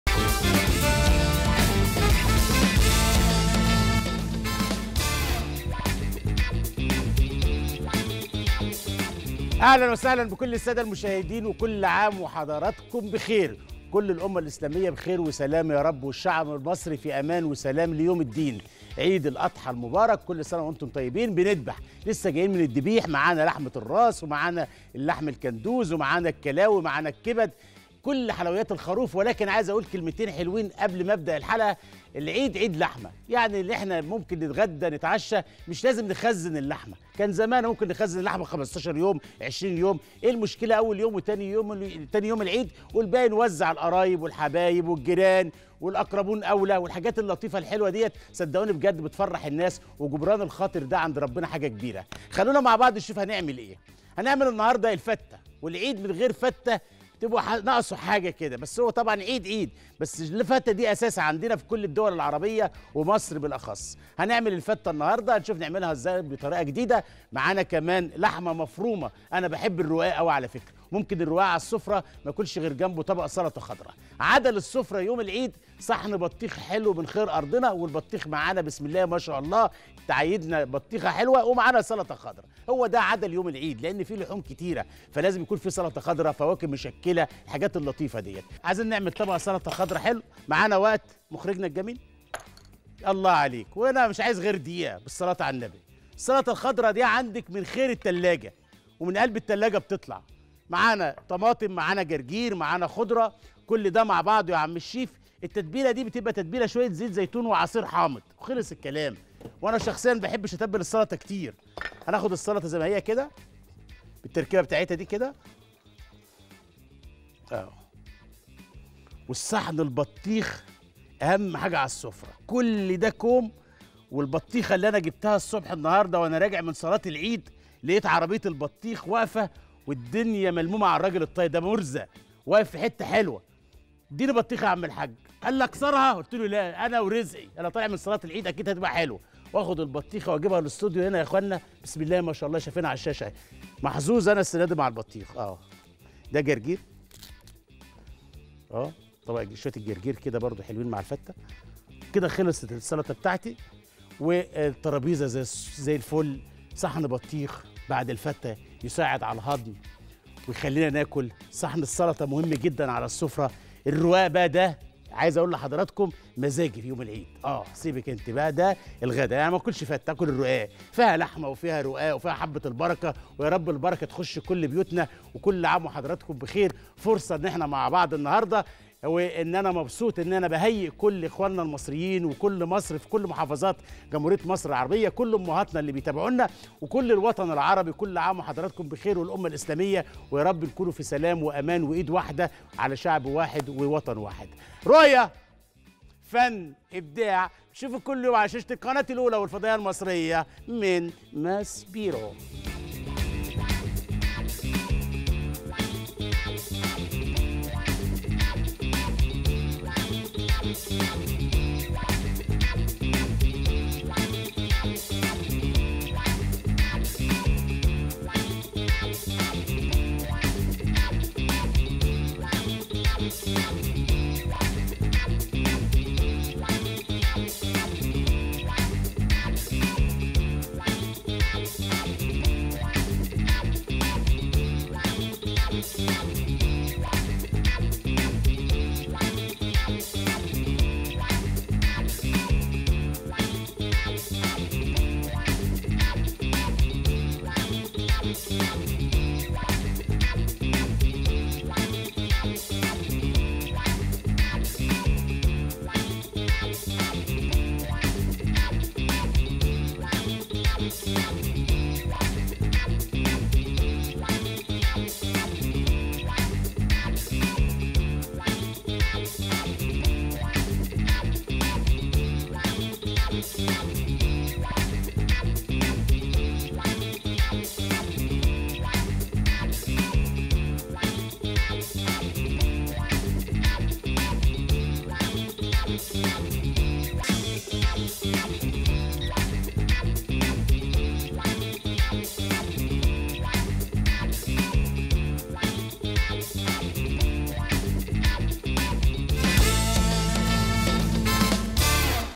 من غير السلطة الخضراء 🥗 السفرة يبقى ناقصها حاجه وليها أصول👌.. مع الشيف المغازي👨‍🍳 هتعملي أحلى سلطة 💪
أكلة أمي | 📌من غير "السلطة خضراء" 🥗 السفرة تبقى ناقصها حاجه بس ليها أصول 👌 ما ينفعش نفوتها.. مع الشيف المغازي 👨‍🍳 هتعملي سلطة زي ما الكتاب بيقول 💪